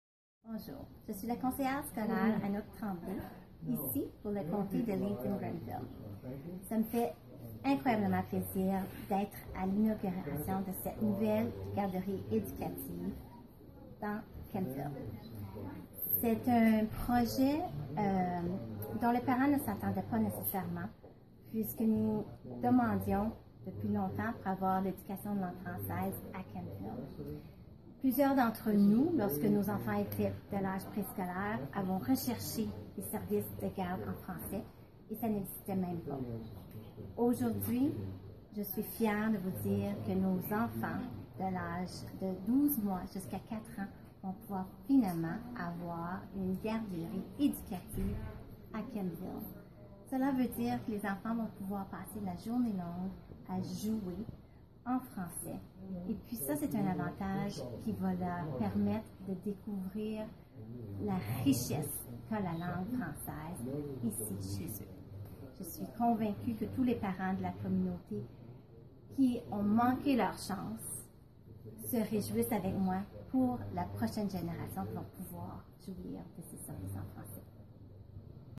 Inauguration de la nouvelle garderie du CECCE à Kemptville
La conseillère scolaire Anouk Tremblay du secteur Lanark, Leeds et Grenville partage sa joie lors de l'inauguration de la nouvelle garderie du CECCE à l'École catholique Sainte-Marguerite-Bourgeoys - pavillon Kemtpville!